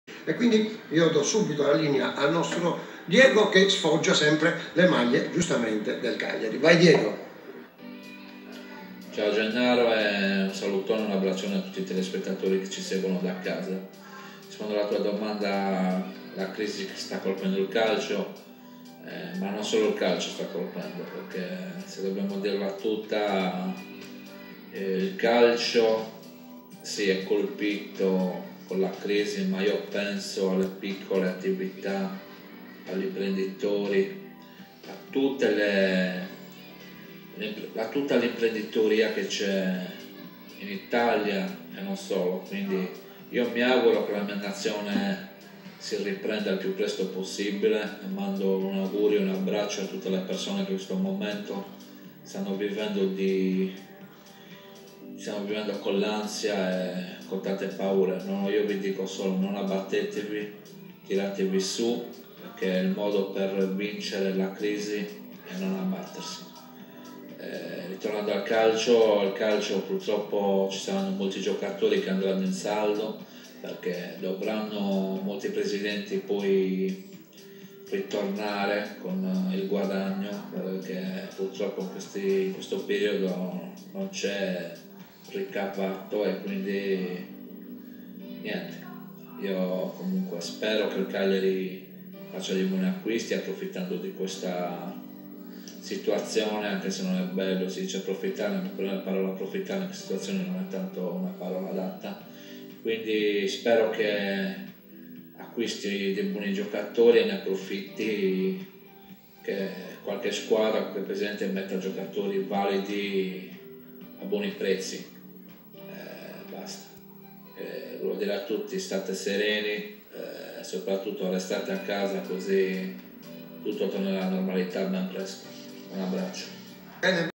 13 Aprile 2020
Con questa crisi , moltissimi giocatori saranno in “ saldo”, secondo voi qual’è la mossa più intelligente che dovrebbe fare il CAGLIARI , approfittarne ? Oppure restare cauto e finito il campionato iniziare a guardarsi intorno?